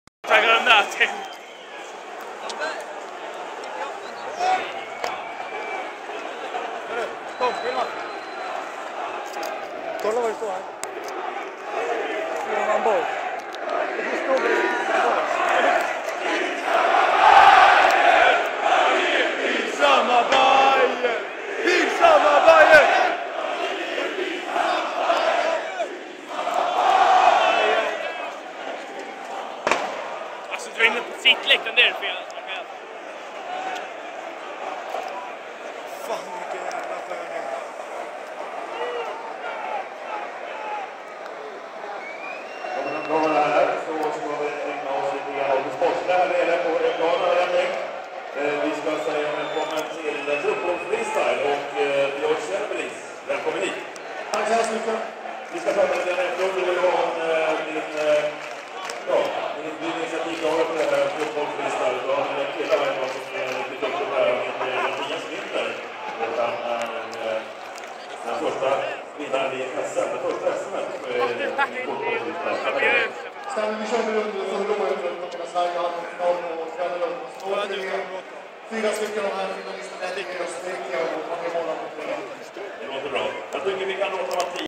Hets på Råsunda
Lite halvtidsstök mellan AIK och Hammarby.